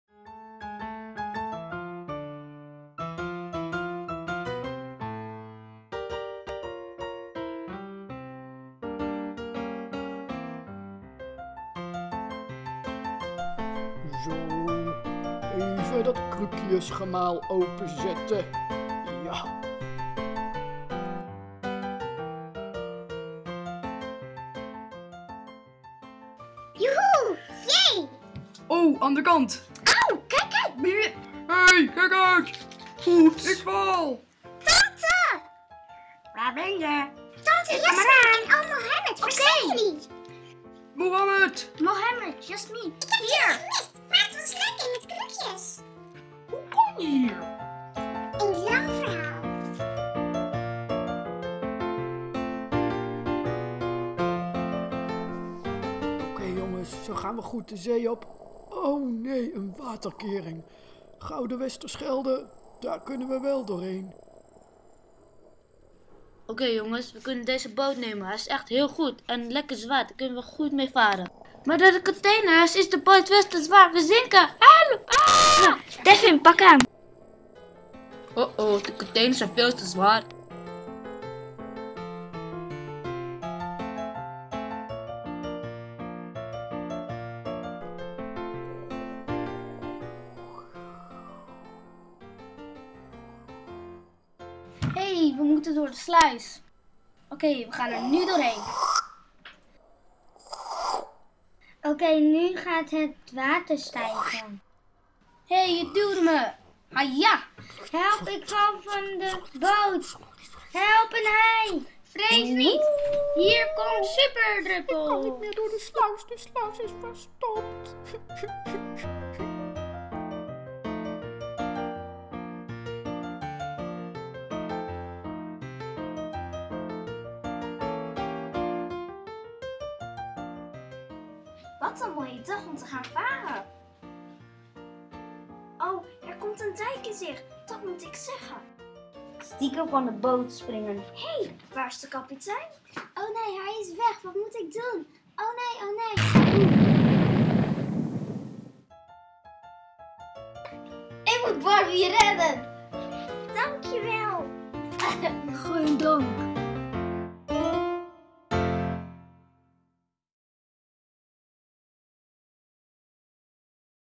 merlijn Groep 6/7b De Waterkringloop
Kunst In Leren, Pier-K. Kinderen uit groep 6/7b van obs Merlijn te Nieuw Vennep verwerken lesstof in een verhaal en daarna in een animatiefilm.